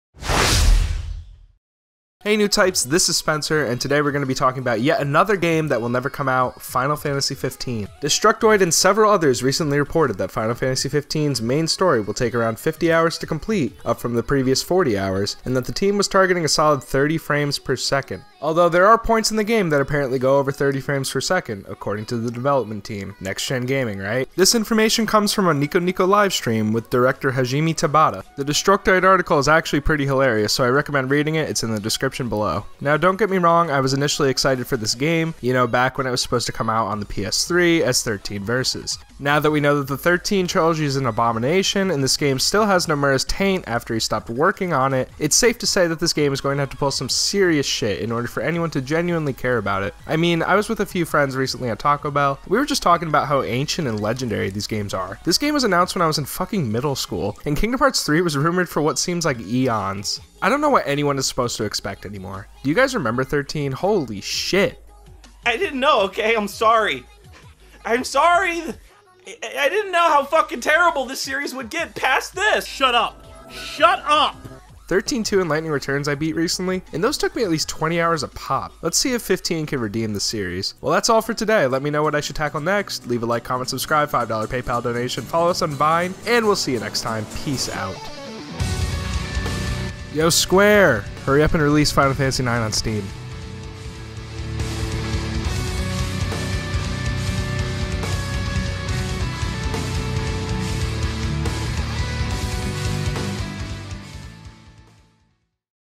FINAL FANTASY XV TARGETS 30 FPS AND 50 HOURS
Can the next gen iteration of Final Fantasy beat out the infamous XIII series?!